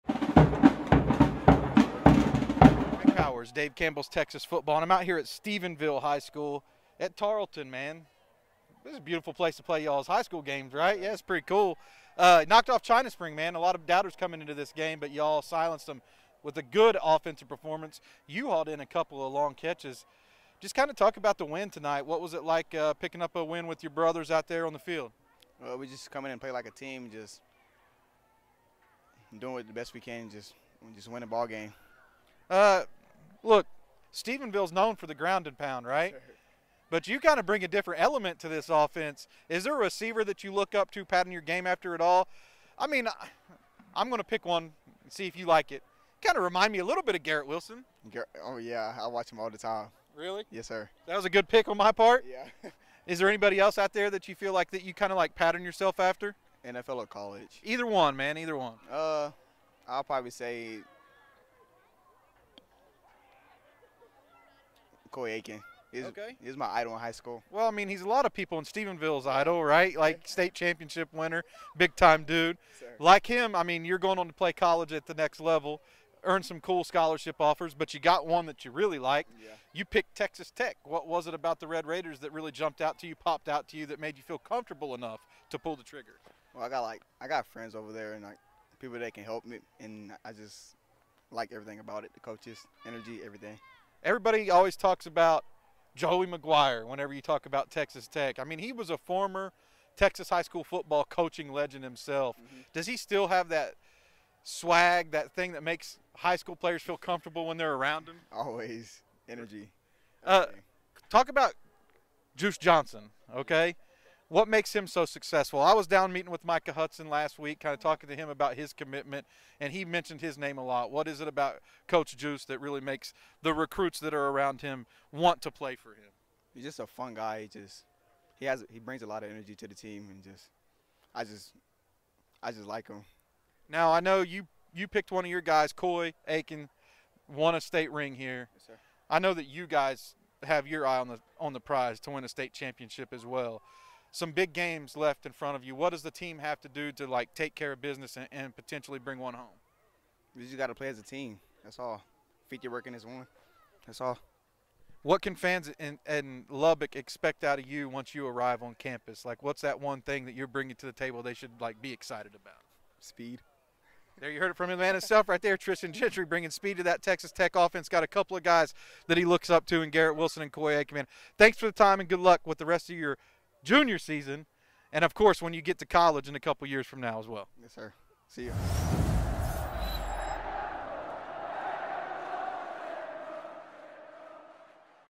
Focus 2025: Stephenville WR Tristian Gentry on Texas Tech Commitment
An interview with 2025 Stephenville wide receiver Tristian Gentry who spoke about a big win over China Spring and his recent commitment to Texas Tech.